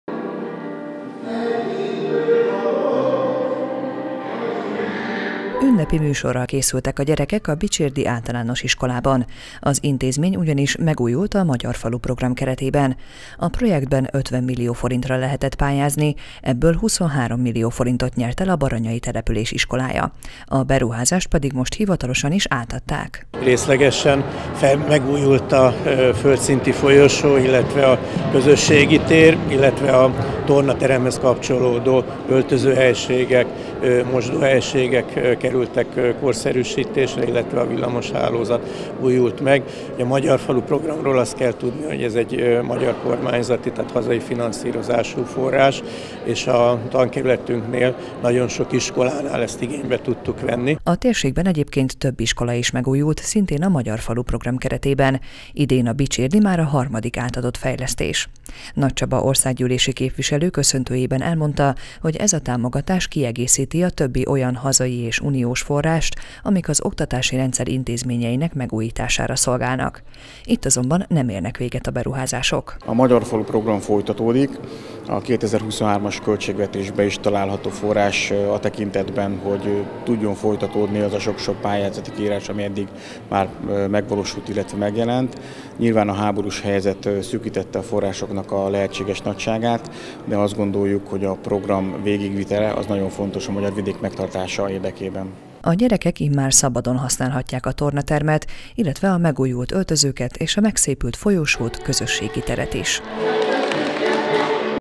Megújult a Bicsérdi Általános Iskola - 23 millió forintból végezték el a beruházást
Megújult a Bicsérdi Általános Iskola - 23 millió forintból végezték el a beruházást a Magyar Falu Programban

23 millió forintból újították fel a Bicsérdi Általános Iskolát. A beruházás a Magyar Falu Program keretében valósult meg. Ennek köszönhetően megszépült a földszinti folyosó és a közösségi tér, illetve a tornateremhez kapcsolódó öltözők és mosdók. Emellett a villamoshálózatot is modernizálták.